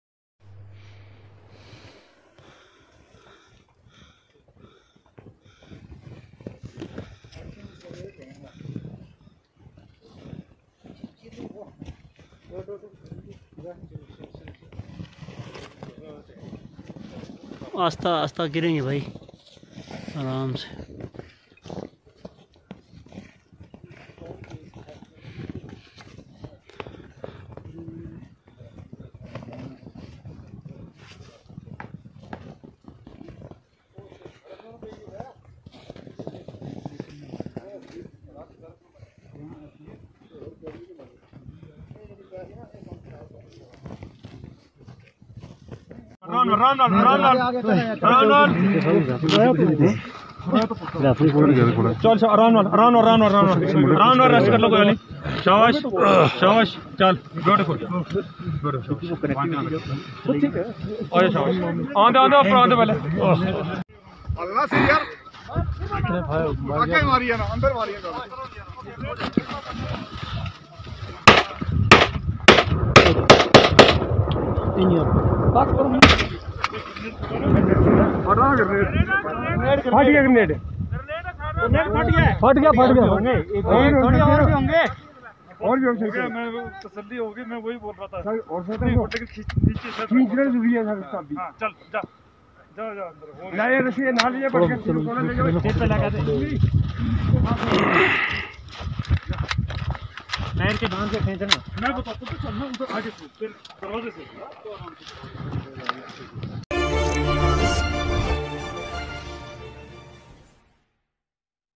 Two Militants Killed, Another One From Shopian Captured Alive At Mughal Road.
Operation chatapani on Mugal road concluded successfully with the elimination of two Pakistani terrorists and one terrorist associate of Shopian arrested in the encounter
The joint operation was conducted by poonch police and 16 Rr
The initial details of recovery is as under

1.AK RIFLES 2 nos
2.AK AMMUNITION 300
3.mag 6
4.UBGL 1
6.HAND GRENADE 5
7.RDX 300 Gm’s
8MOBILE 2
9.THURAYYA SET
REGARDS